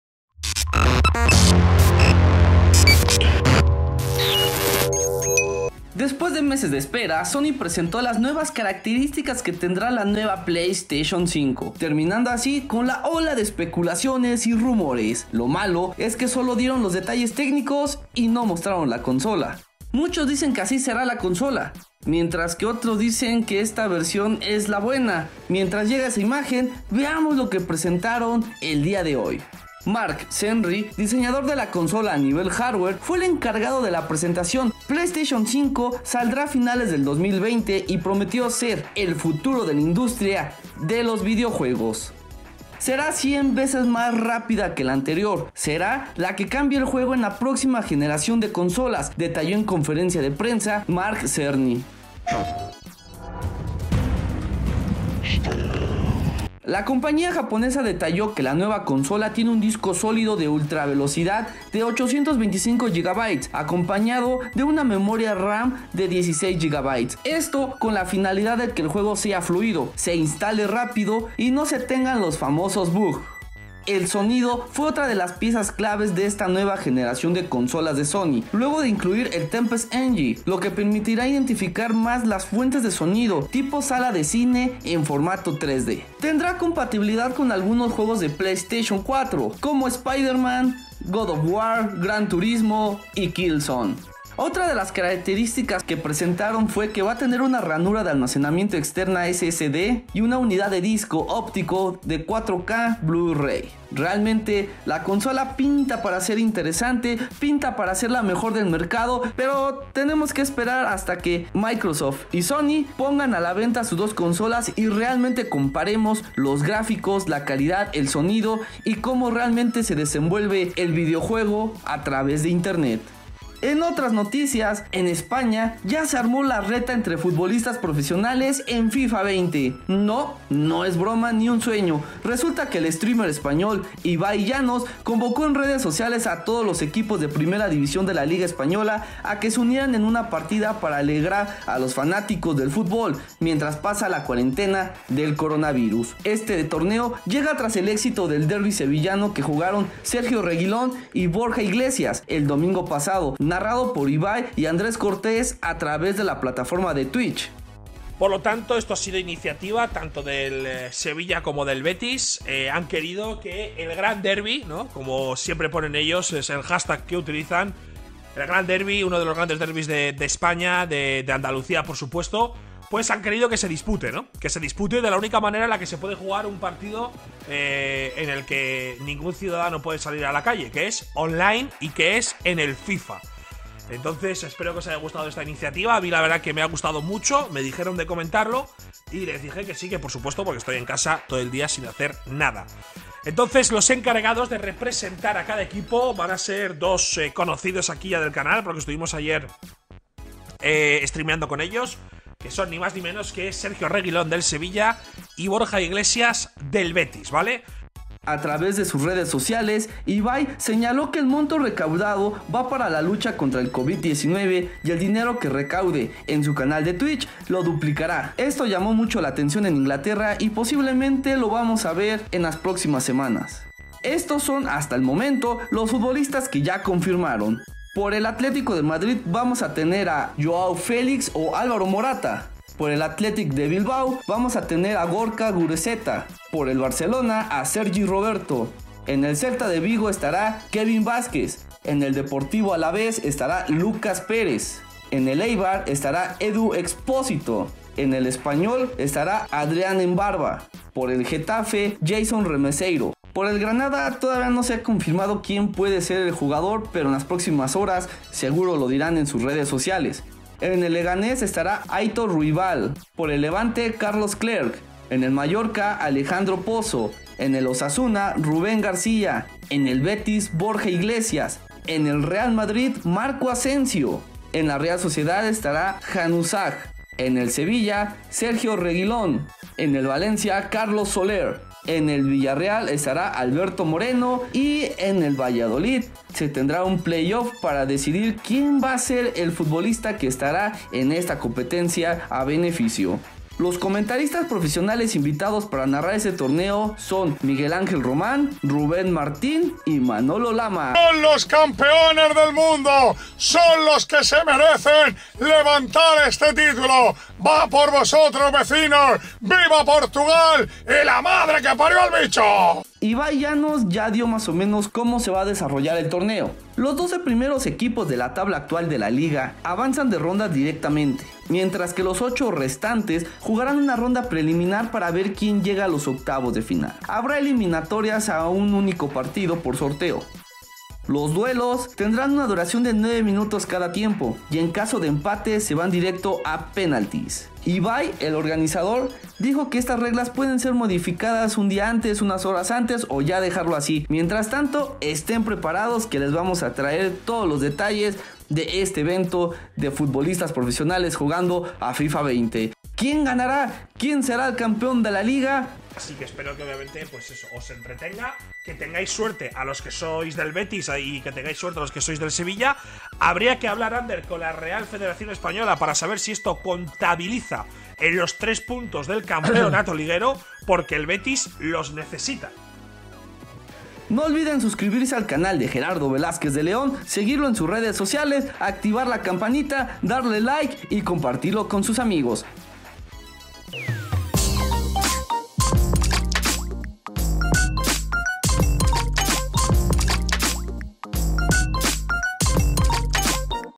La reta en FIFA 20 de los jugadores de la liga española - Conoce los detalles del PlayStation 5
En España ya se armó la reta entre futbolistas profesionales en FIFA 20. No, no es broma. Resulta que el streamer español, Ibai Llanos, convocó en redes sociales a todos los equipos de primera división de la liga española a que se unieran en un partida para alegrar a los fanáticos del futbol, mientras pasa la cuarentena por el coronavirus.